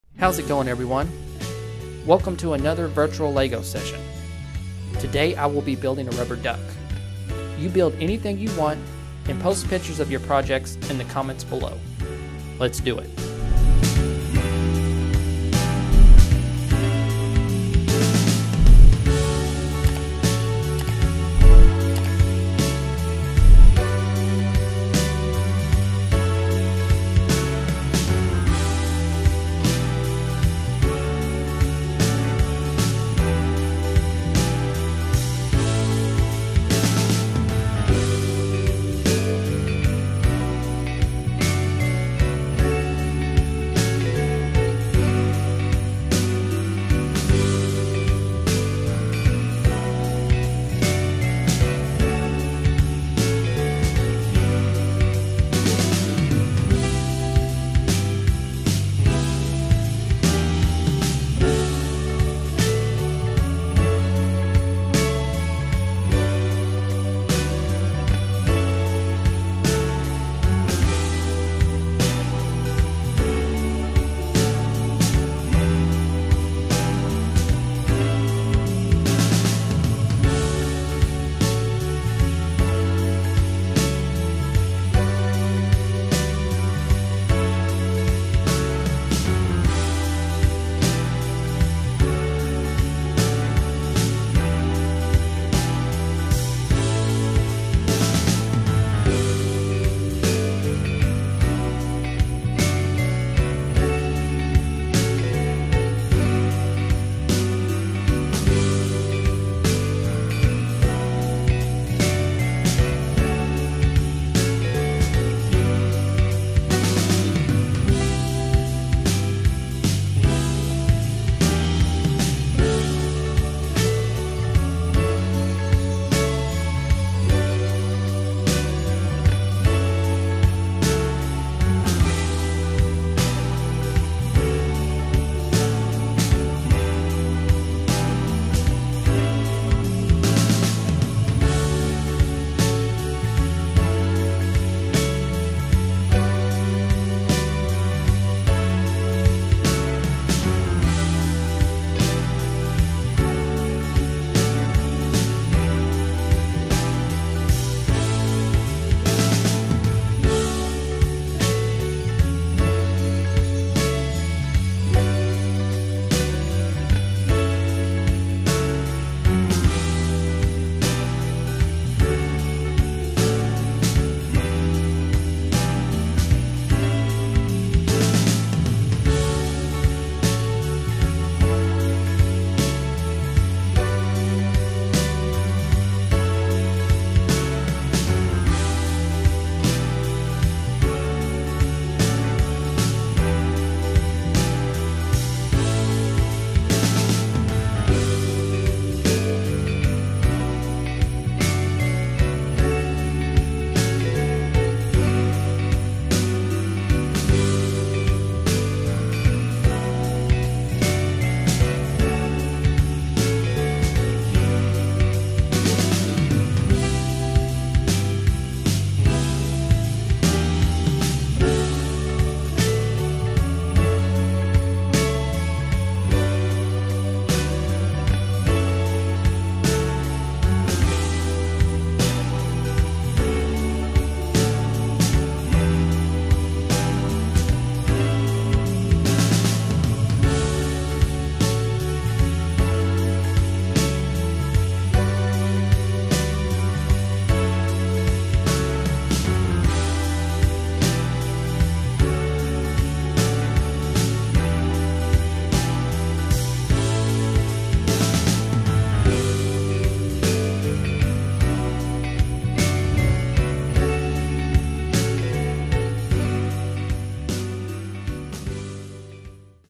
Let's Build It! Build This Rubber Duck Out of Legos!
Join us at home and build this rubber duck out of Legos!